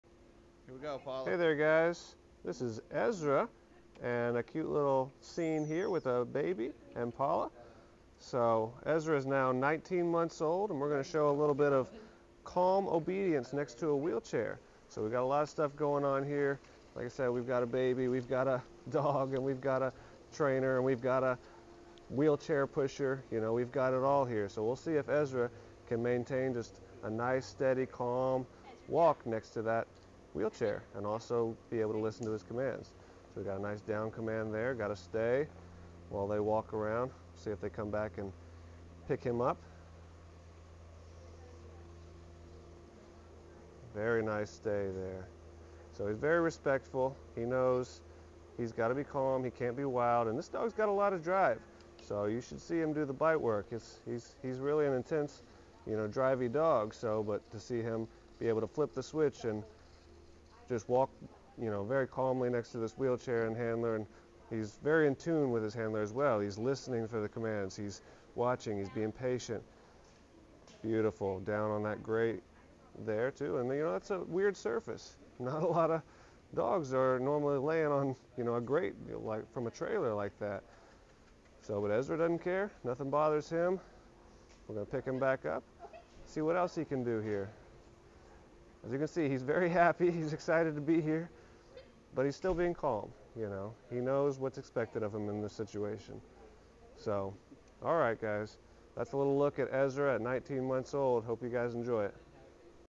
Super Dog In Development @ Protection Dog Sales "Ezra" 19 Mo's Sable German Shepherd Obedience
Beautiful, happy, fun and powerful, yet safe, personal protection dog for sale.

Protection Dog Sales is the #1 World Leader in the breeding, development and training of "Home Raised" personal protection dogs.